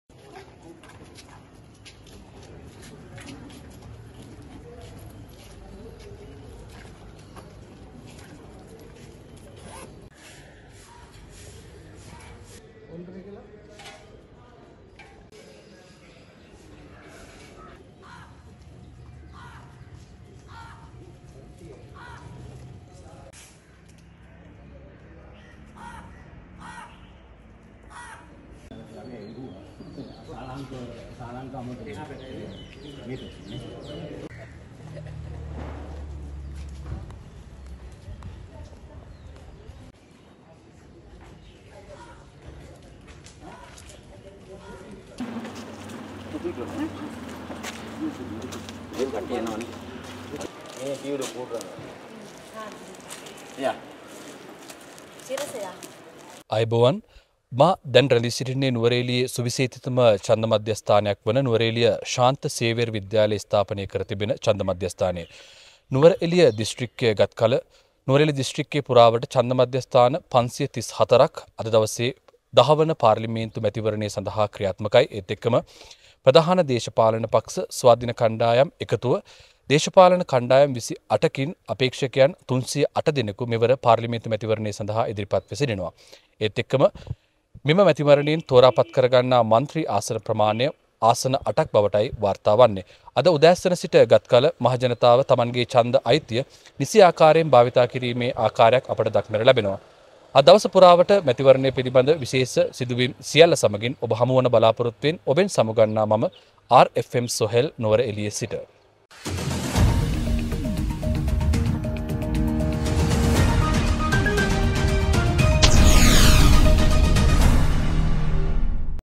මෙන්න නුවරඑළියේ දැන් ප්‍රතිඵලය | සාමයට බාධාවක් නොවී සිද්ධවෙන නුවරඑළියේ ඡන්ද විමසීමේ දැන් තත්ත්වය
මෙන්න නුවරඑළියේ දැන් ප්‍රතිඵලය | කට්ටිය පෝලිමට ඇවිත් ඡන්දය දායි | සාමයට බාධාවක් නොවී සිද්ධවෙන නුවරඑළියේ ඡන්ද විමසීමේ දැන් තත්ත්වය